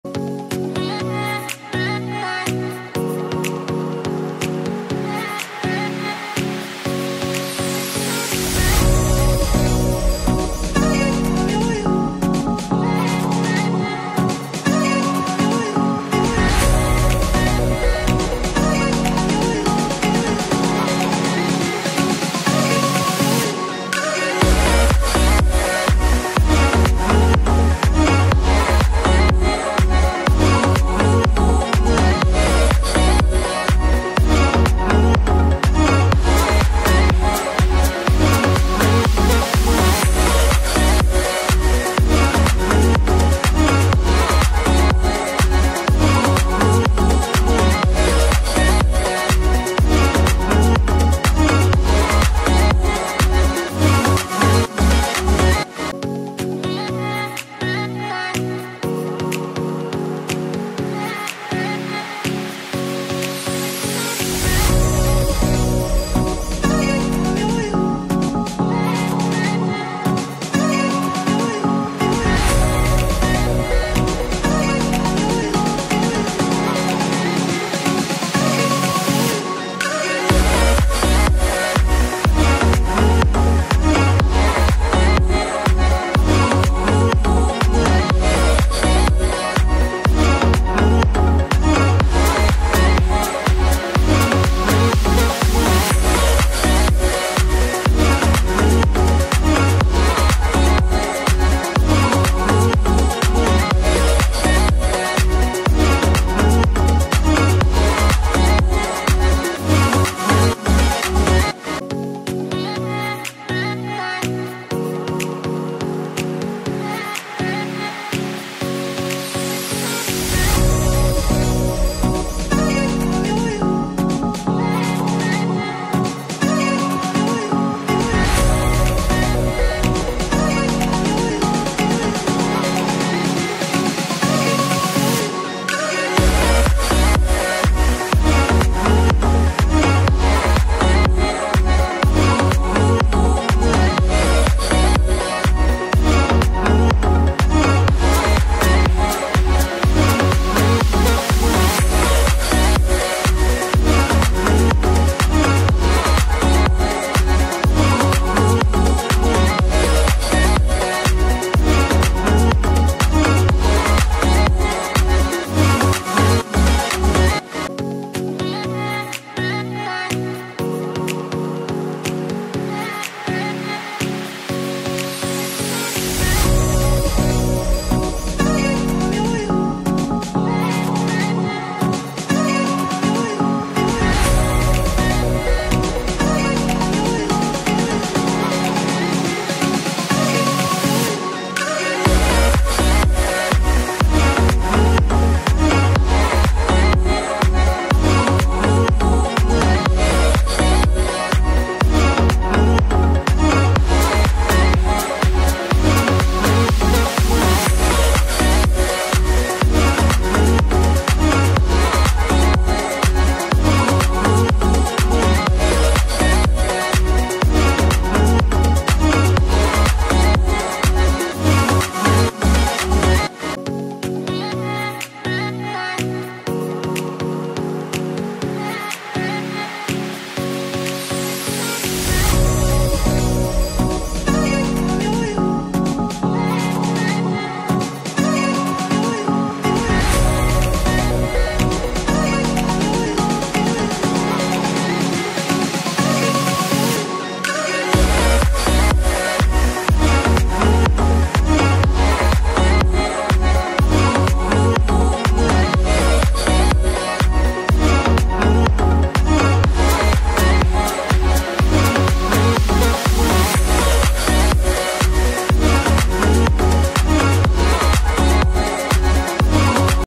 Northeast United Fc ❤ Team Recent Conform Players Signing 2021-2022 | NEUFC Conform Players 2021-22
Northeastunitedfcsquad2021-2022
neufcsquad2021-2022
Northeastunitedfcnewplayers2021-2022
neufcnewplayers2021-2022

All credit to Northeast United Fc ❤🙏